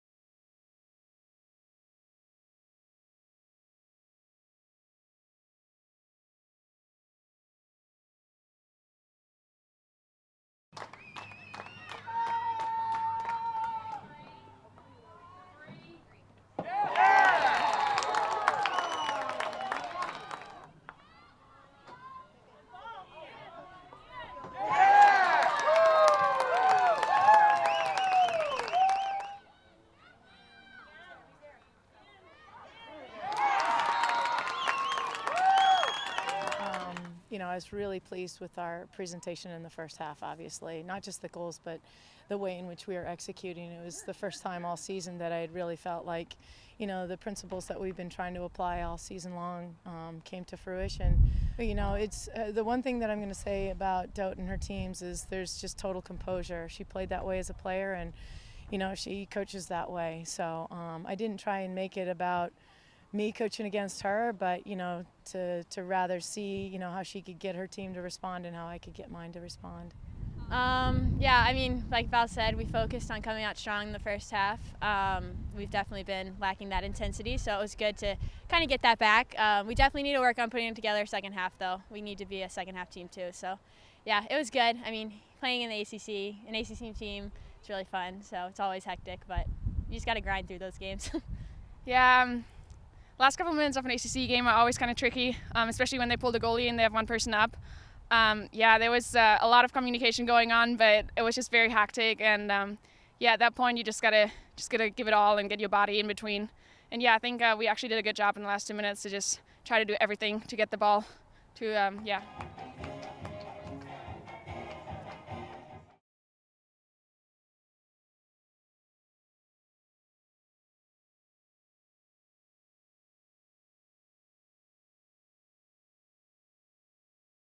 Wake Forest Field Hockey: Wake Forest 3, Boston College 2
The seventh-ranked Wake Forest field hockey team outlasted No. 11 Boston College for a 3-2 win on Sunday at Kentner Stadium. The Demon Deacons improved to 3-0 in ACC play.